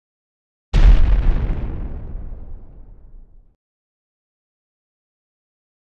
Blast Wave
In this video I created an extensive explosion combined with a shockwave.